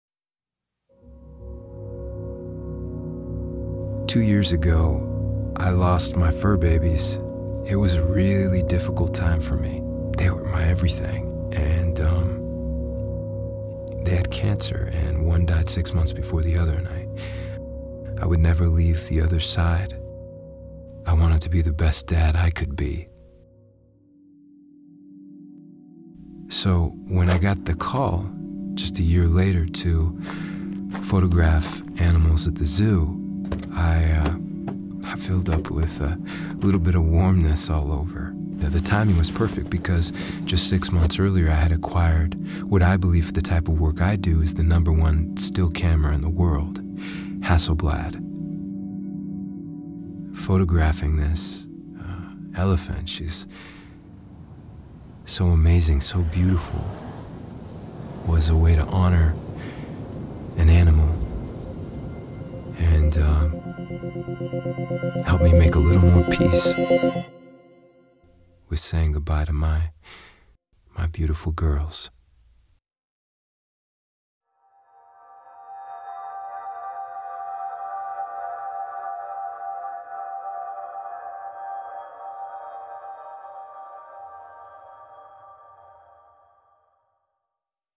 Choosing Hasselblad
One of the key ingredients I strive to bring to all of my work creatively is authenticity. I hope you feel it emanates from this sixth vlog style share.

I’d like to thank the Director/DP who gave me the opportunity to photograph this campaign. The majority of my new work is coming through word of mouth. I feel extremely grateful to have developed a beautiful circle of friends and clients that believe in me.

As always, thank you for your continued support. I appreciate each and every one of you. If you’d like to see the billboard ‘in print’ you can view it